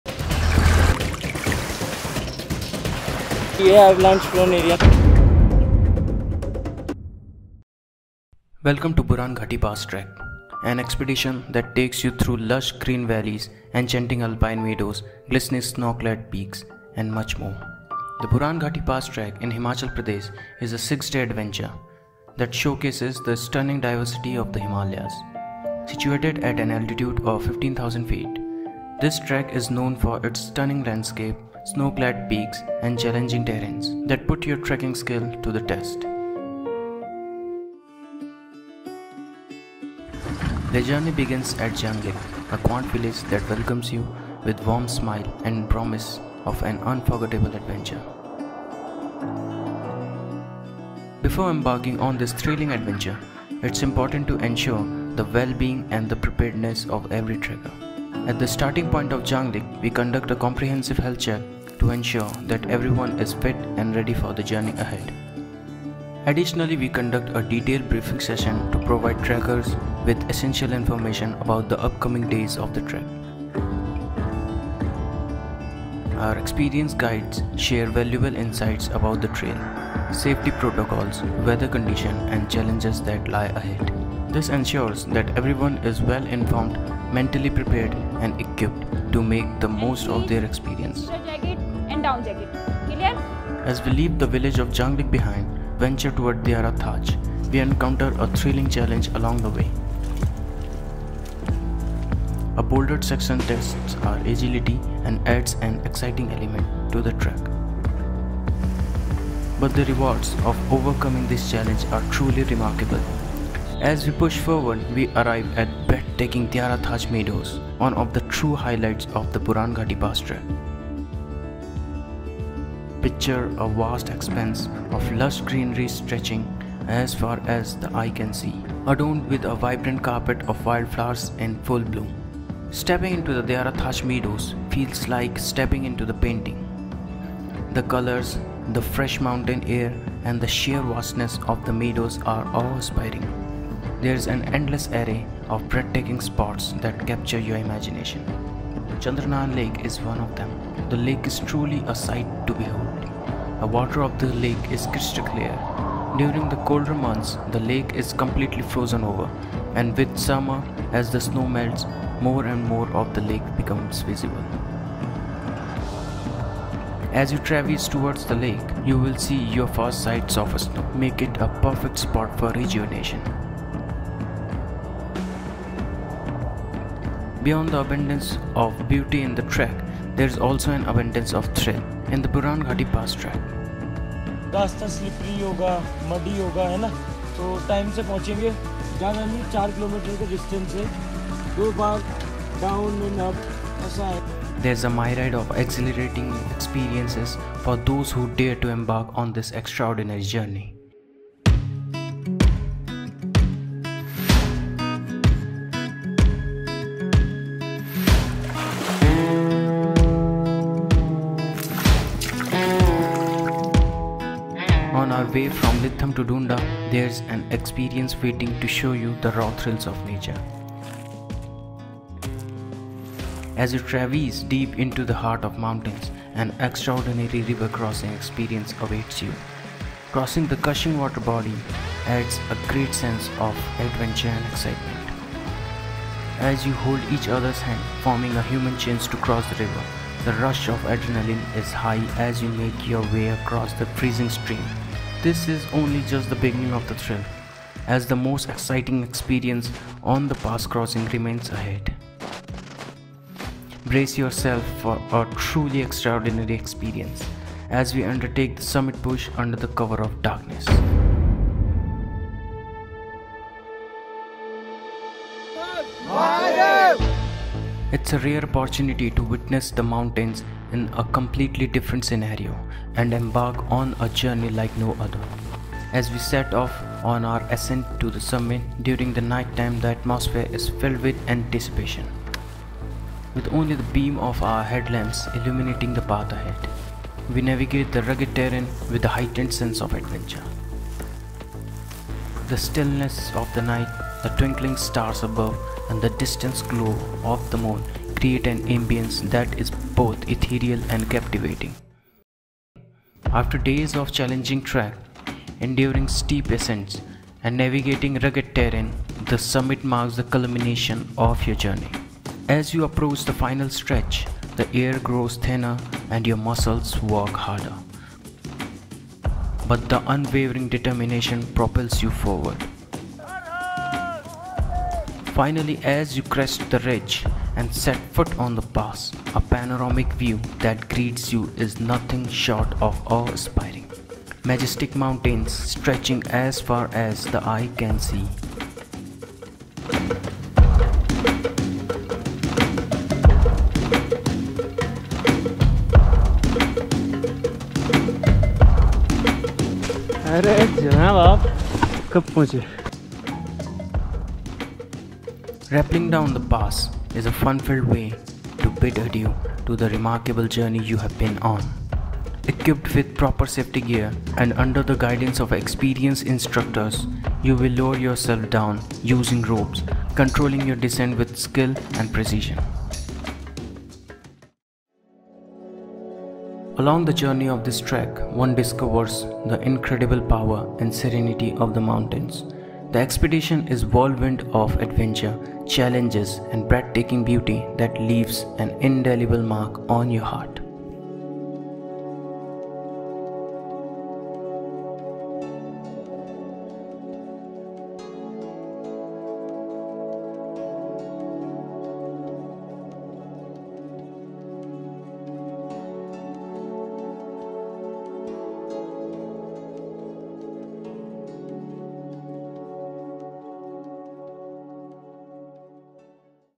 BURAN GHATI PASS - Route connecting the Baspa valley of Kinnaur to Pabbar valley of Shimla
Join us on an unforgettable adventure as we take you through the mesmerizing Buran Ghati Pass Trek. In this exhilarating YouTube video, our expert trekking guides will lead you on a virtual expedition, showcasing the breathtaking landscapes and the thrilling adventures that define this offbeat trail. Get ready to experience the Buran Ghati Pass Trek from the comfort of your screen!